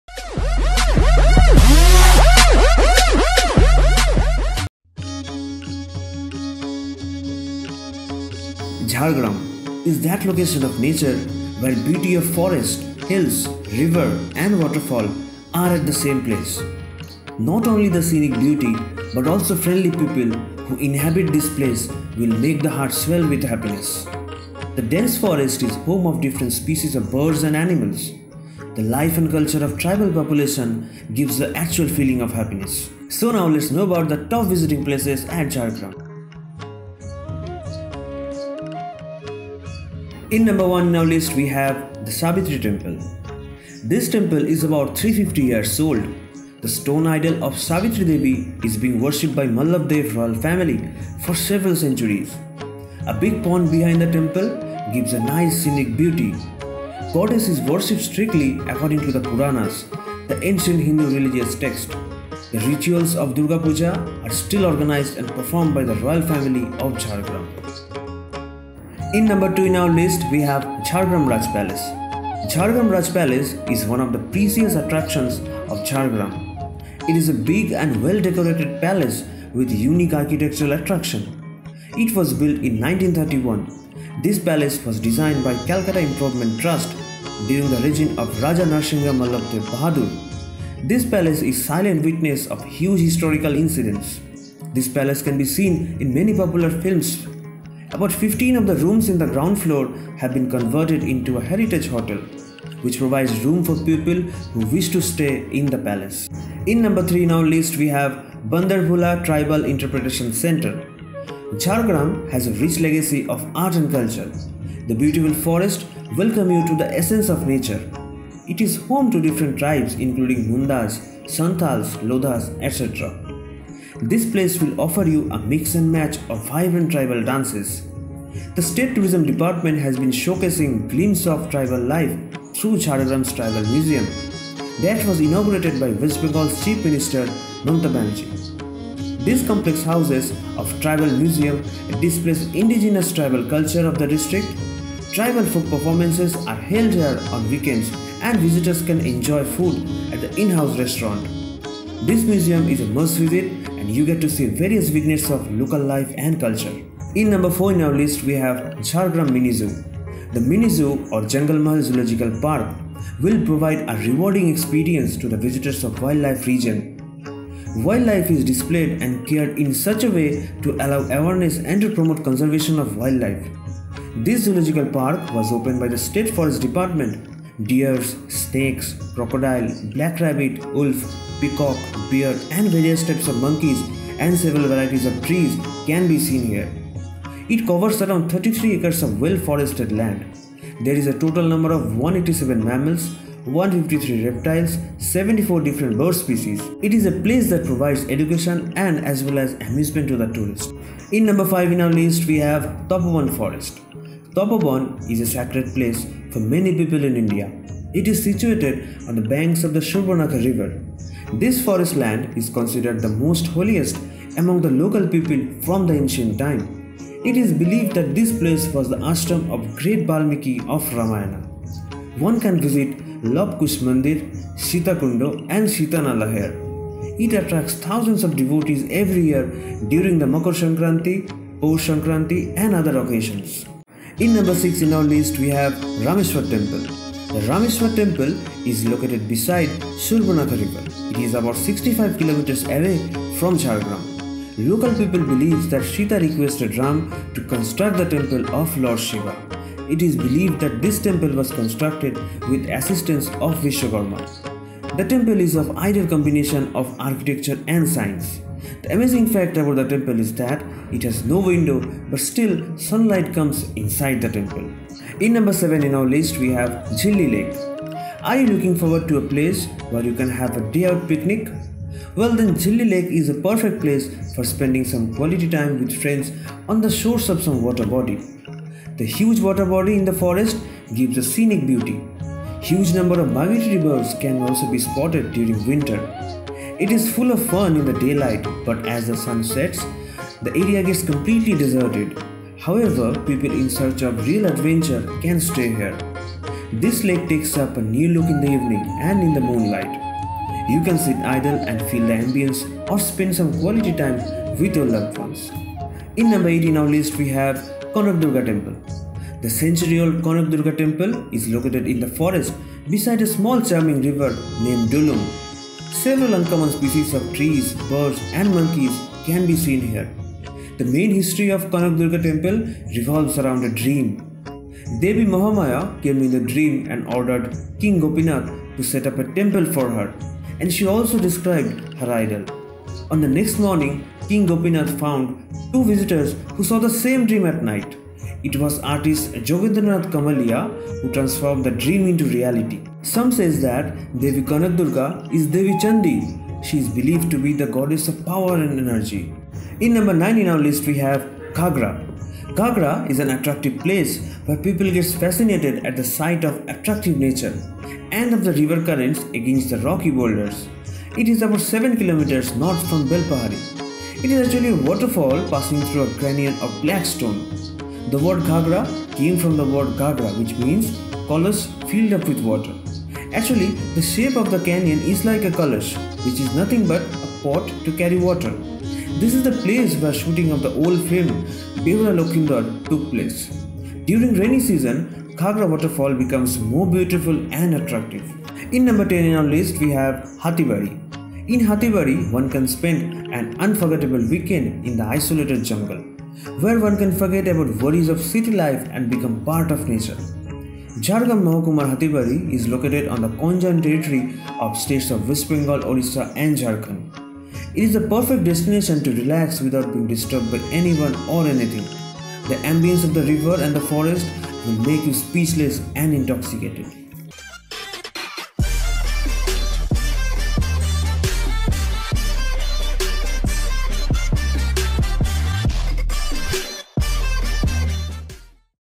Jhargram || Experience West of West Bengal || Top Weekend Gateways
Experience Jhargram "The Queen of forest" into the West of West Bengal

Jhargram is that location of nature where beauty of forests, hills, river and waterfall are at the same place. In Jhargram Not only the scenic beauty but also friendly people who inhabit this place will make the heart swell with happiness. The dense forests is home of different species of birds & animals. Experience The life and culture of the tribal population gives the actual feeling of happiness. Experience The Top Weekend Gateways of Jhargram in this video

Follow our channel for more upcoming videos